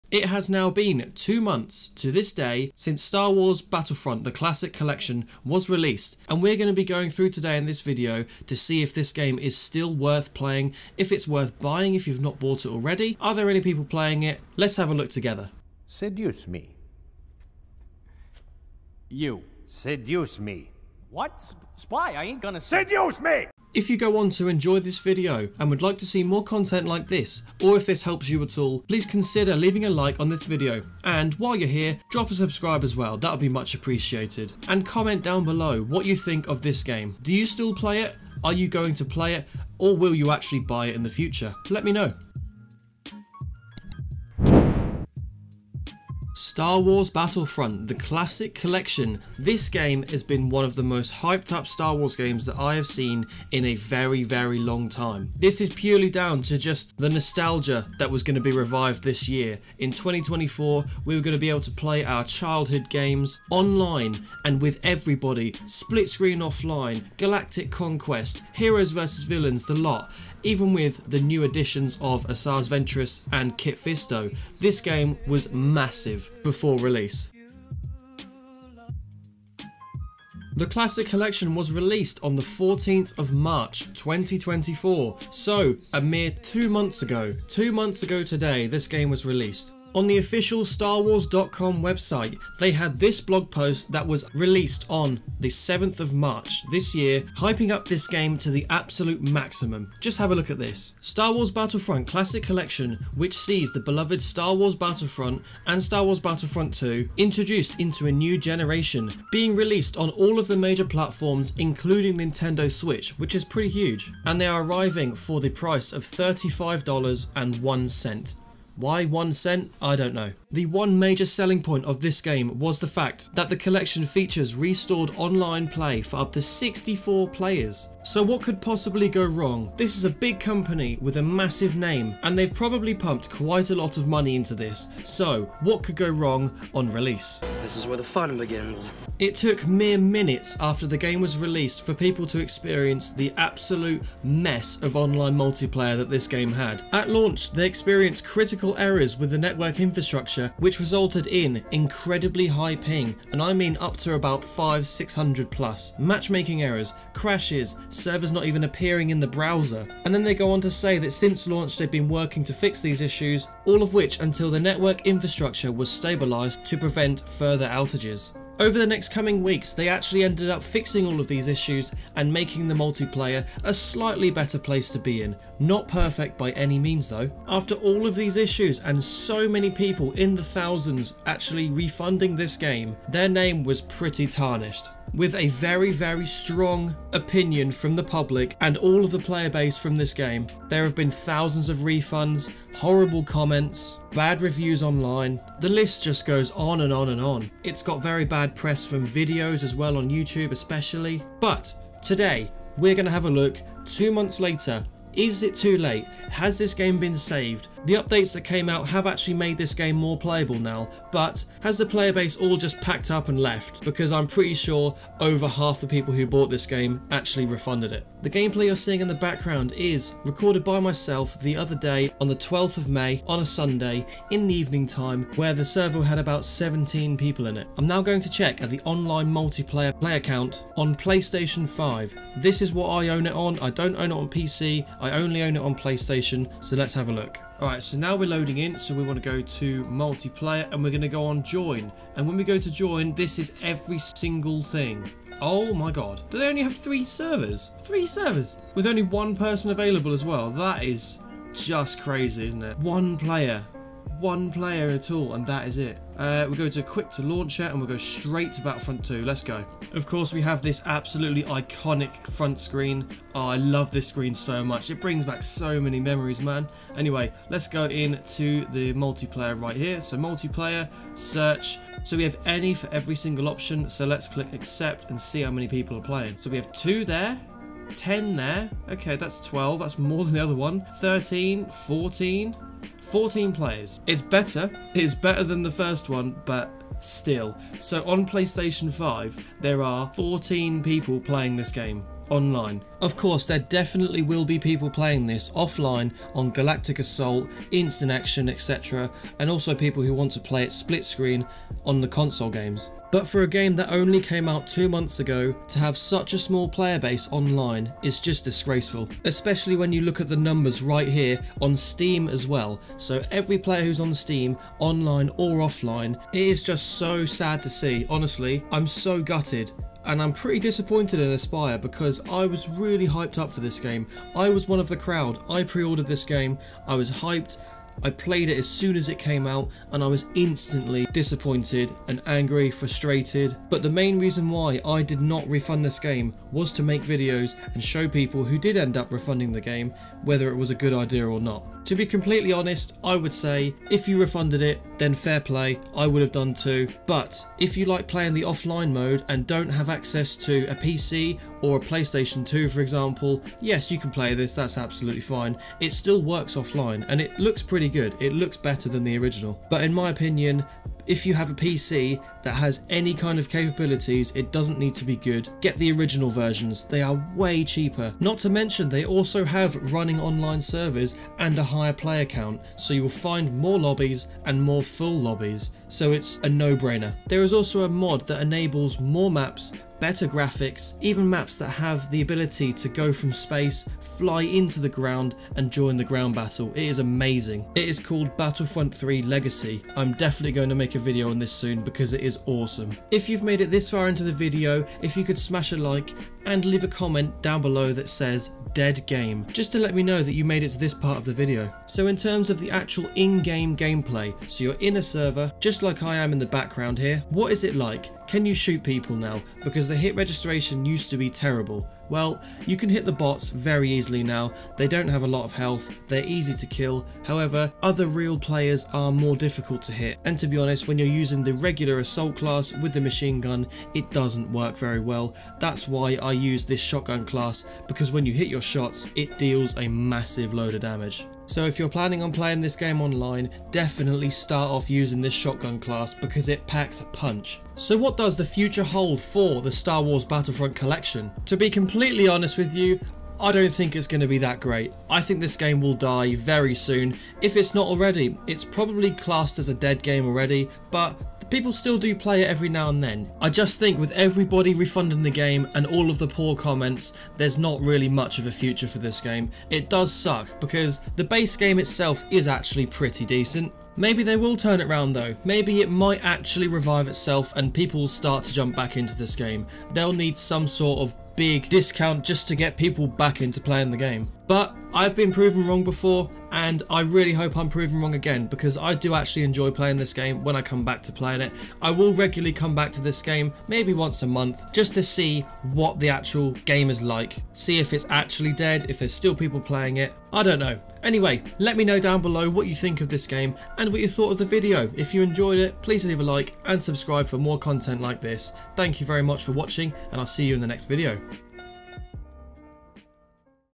Is STAR WARS Battlefront Classic Collection Worth Playing 2 MONTHS LATER? Should You Buy It?!
Is Star Wars Battlefront Classic Collection worth playing on PC, Xbox, PS5 and Switch 2 months after release? Is it too late?

TIMESTAMPS:
00:00 Intro
00:45 Release & Reviews
03:37 Player Count
05:16 Is it Worth Buying?
06:41 State of Online Multiplayer
07:17 Future of the Game